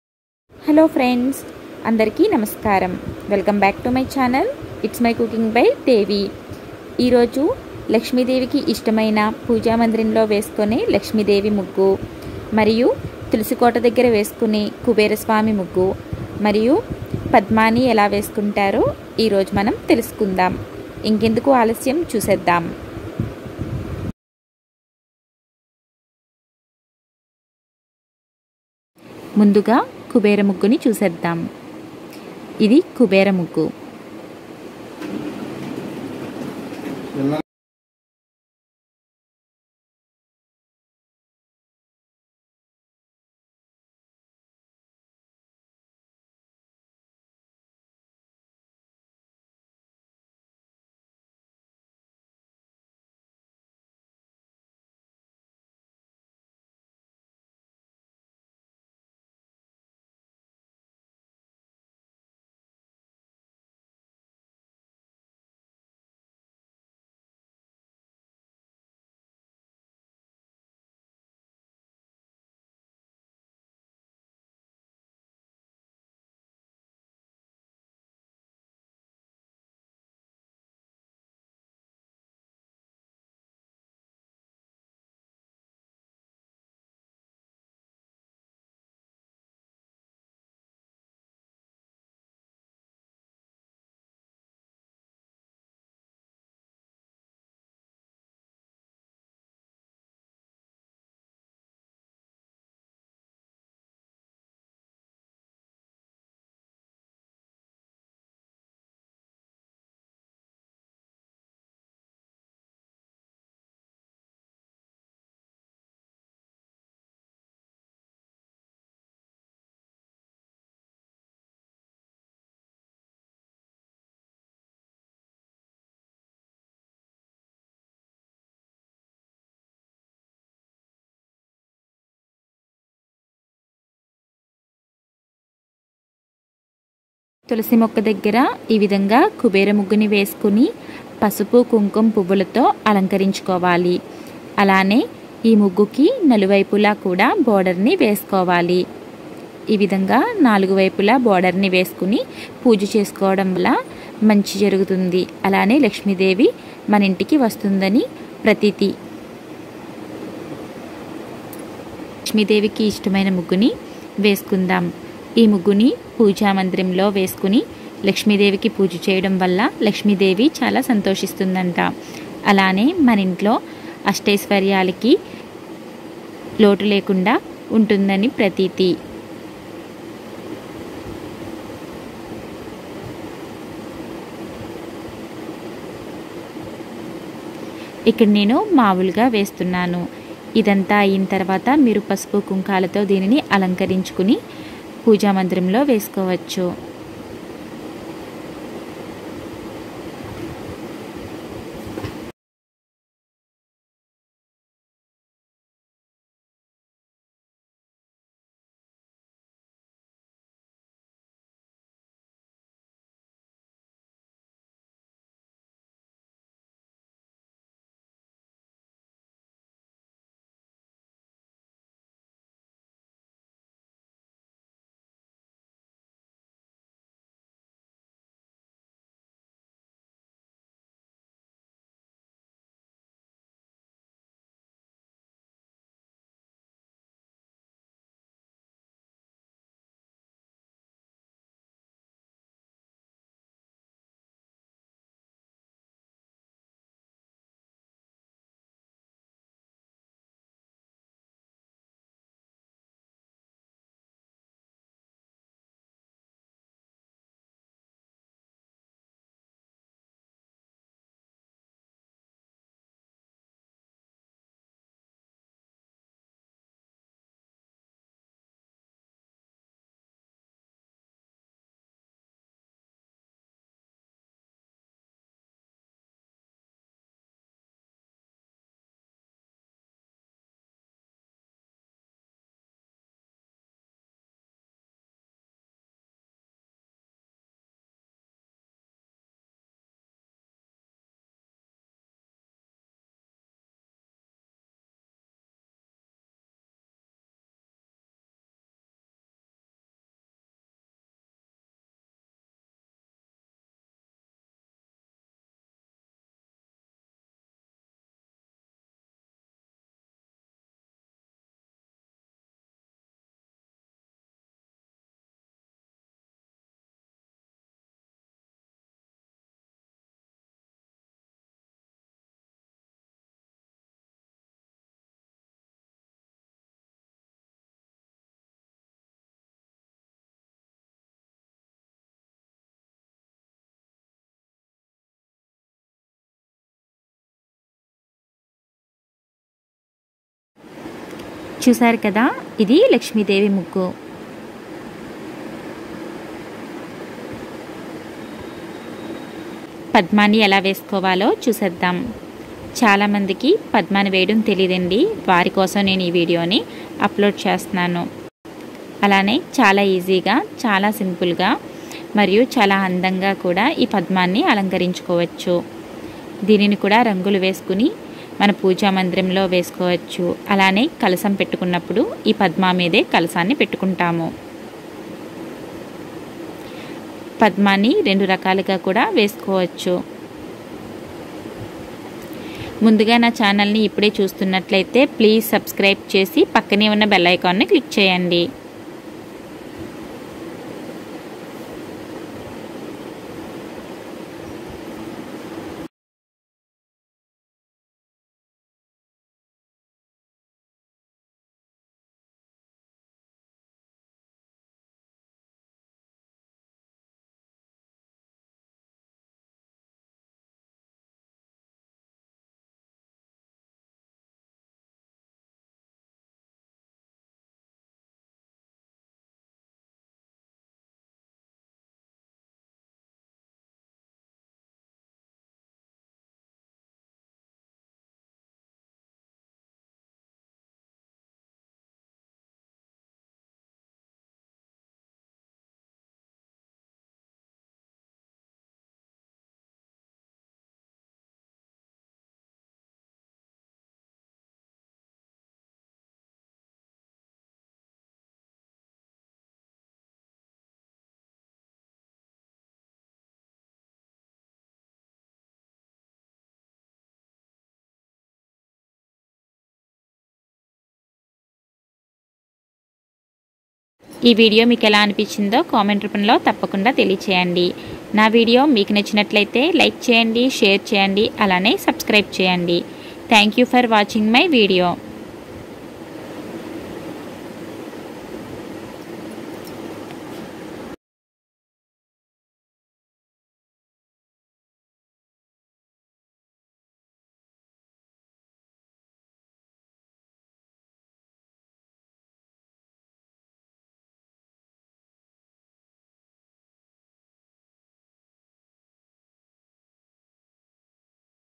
పూజ మందిరంలో వేసే లక్ష్మీదేవి ముగ్గు# తులసి కోట దగ్గర వేసే కుబేర ముగ్గు# పద్మ ముగ్గు#
Lakshmi Devi rangoli/ Kubera muggu/padhma muggu.simple rangoli designs.
rangoli disigns for bigners..